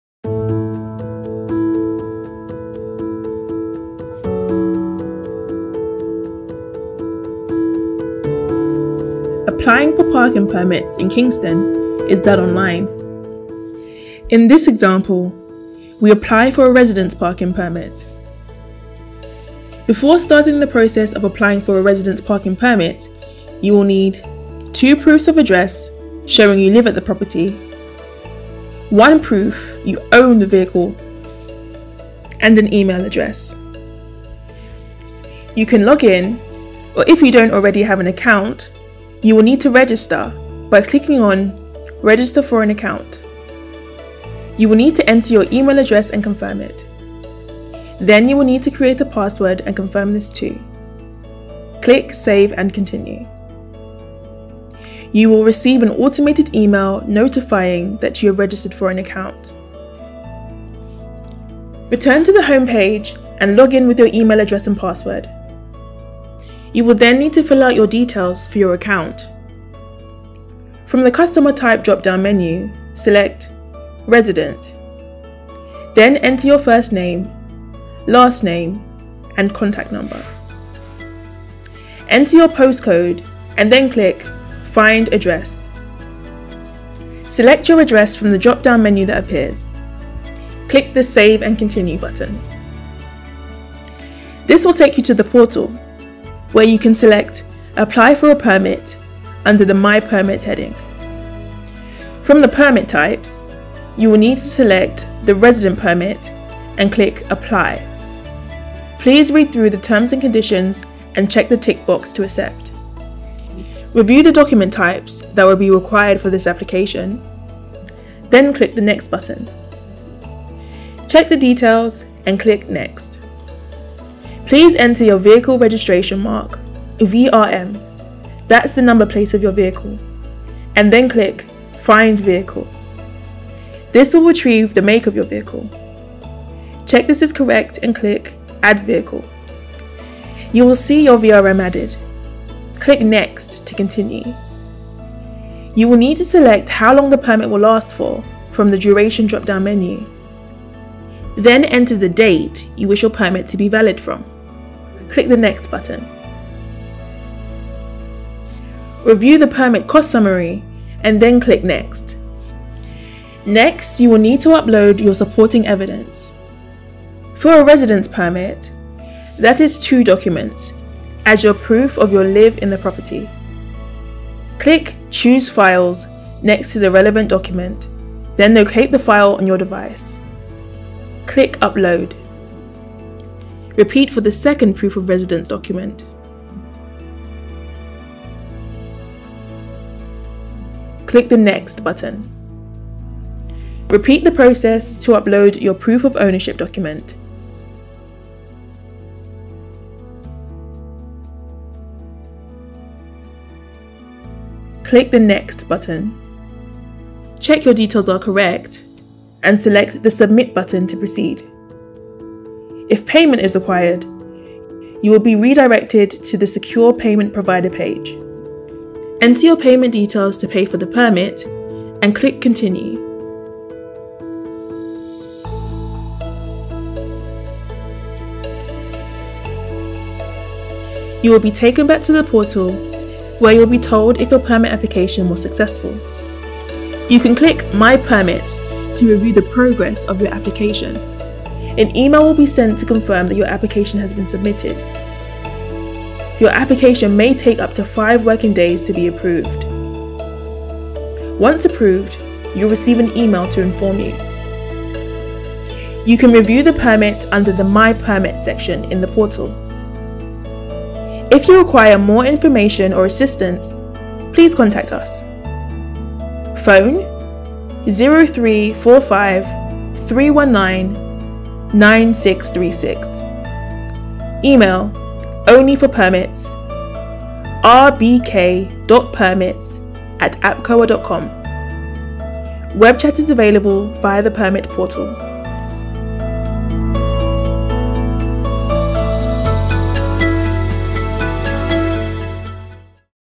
Applying for residents parking permits
How to apply for a parking permit as a resident in Kingston